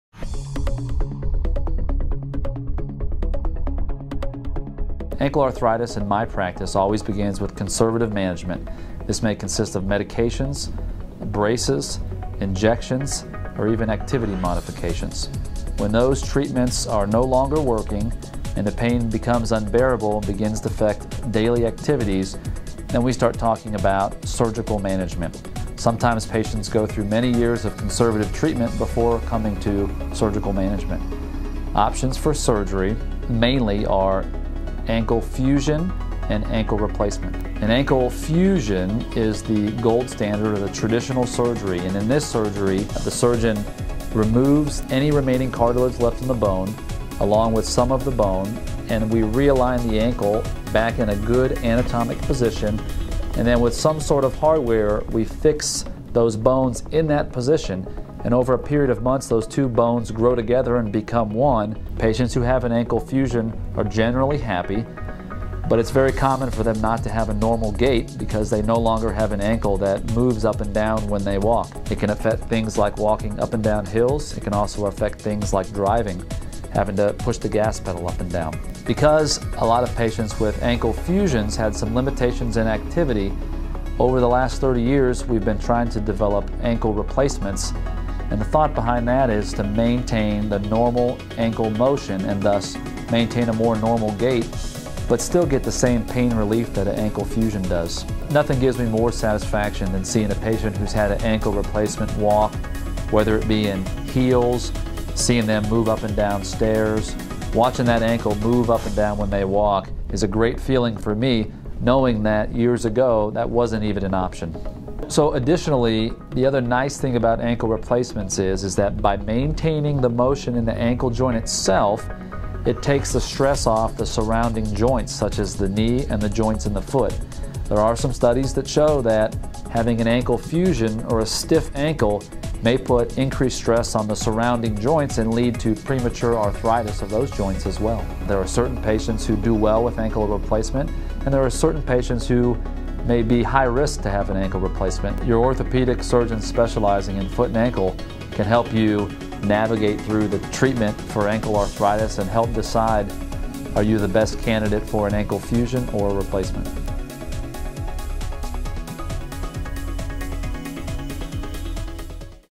Dr Jason Lake Ankel Replacement Video
Dr. Lake is a board certified orthopedic foot and ankle surgeon.